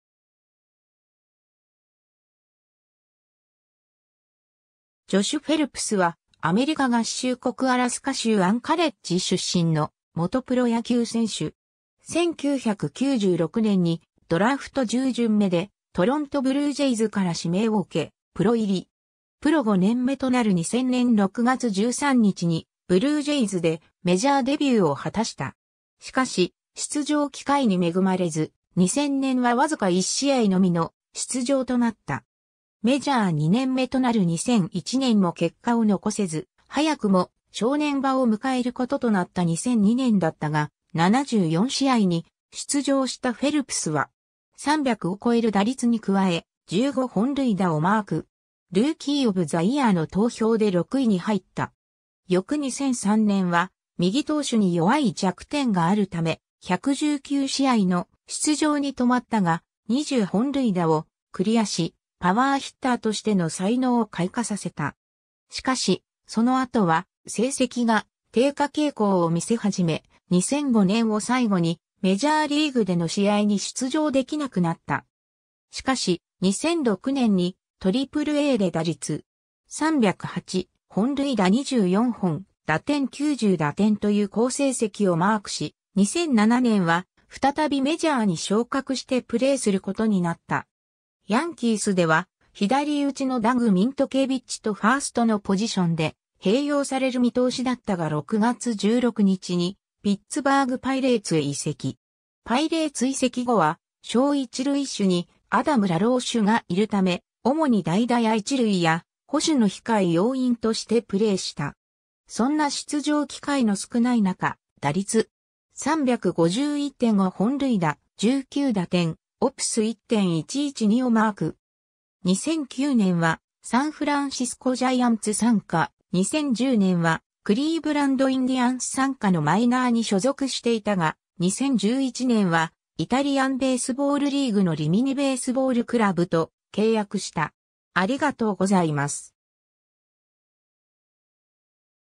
ジョシュ・フェルプス
タンパベイ・デビルレイズの選手
ブリッジポート・ブルーフィッシュの選手
ジョシュ・フェルプス（Joshua Lee Phelps, 1978年5月12日 - ）は、アメリカ合衆国アラスカ州アンカレッジ出身の元プロ野球選手（一塁手）。
 1996年に、ドラフト10巡目でトロント・ブルージェイズから指名を受け、プロ入り。
プロ5年目となる2000年6月13日に、ブルージェイズでメジャーデビューを果たした。
しかし、出場機会に恵まれず、2000年はわずか1試合のみの出場となった。
メジャー2年目となる2001年も結果を残せず、早くも正念場を迎える事となった2002年だったが、74試合に出場したフェルプスは.300を超える打率に加え、15本塁打をマーク。
ルーキー・オブ・ザ・イヤーの投票で6位に入った（2002年のルーキー・オブ・ザ・イヤーは、元チームメイトのエリック・ヒンスキー）。
翌2003年は、右投手に弱い弱点がある為、119試合の出場に留まったが、20本塁打をクリアし、パワーヒッターとしての才能を開花させた。
しかし、その後は成績が低下傾向を見せ始め、2005年を最後にメジャーリーグでの試合に出場できなくなった。
 しかし、2006年にAAAで打率.308、本塁打24本、打点90打点という好成績をマークし、2007年は、再びメジャーに昇格してプレイする事になった。
ヤンキースでは、左打ちのダグ・ミントケイビッチとファーストのポジションで併用される見通しだったが6月16日にピッツバーグ・パイレーツへ移籍。
 パイレーツ移籍後は、正一塁手にアダム・ラローシュがいるため、主に代打や一塁や捕手の控え要員としてプレーした。
そんな出場機会の少ない中、打率.351・5本塁打・19打点・OPS1.112をマーク。
 2009年はサンフランシスコ・ジャイアンツ傘下、2010年はクリーブランド・インディアンス傘下のマイナーに所属していたが、2011年はイタリアンベースボールリーグのリミニ・ベースボールクラブと契約した。